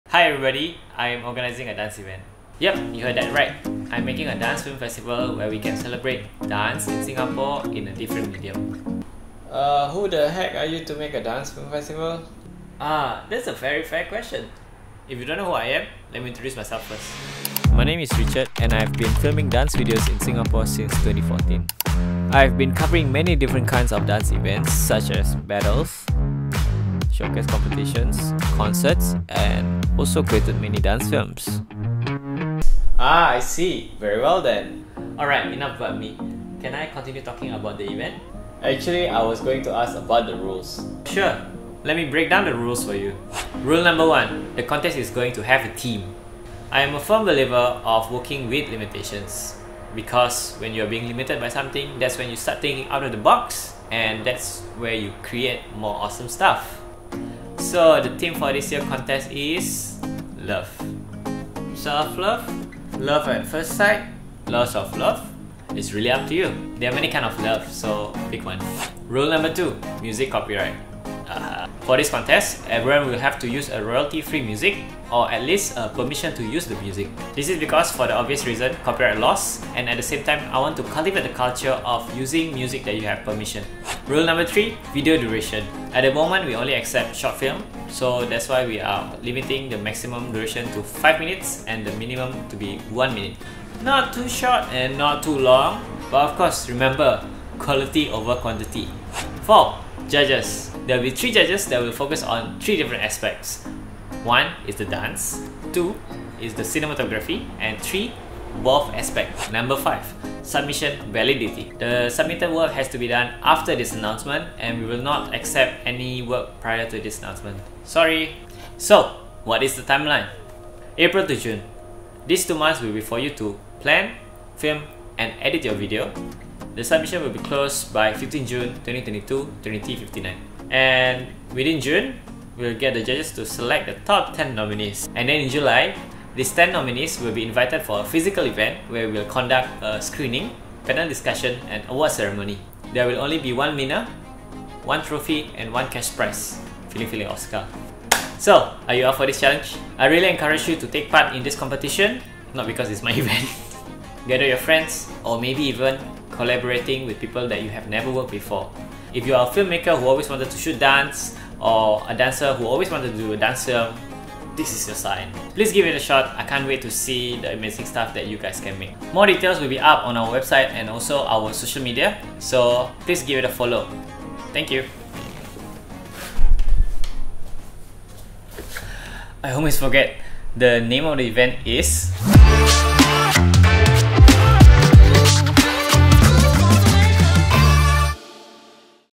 I Am Organizing A Dance Event.
Welcome to Lion City Dance Film Festival! I am Richard, the founder of @rpprods 👋

This festival was created to give our local talents 🇸🇬 a platform to push themselves further in their crafts – both in dance and in filmmaking. As a dance filmmaker myself, I was immensely inspired to create this platform because of the amazing works being put out during these trying times.

As such, I would like to welcome everyone to participate in this Dance Film Festival 🎞 Let's push our boundaries even more and create amazing local works that we can all be proud of 🤩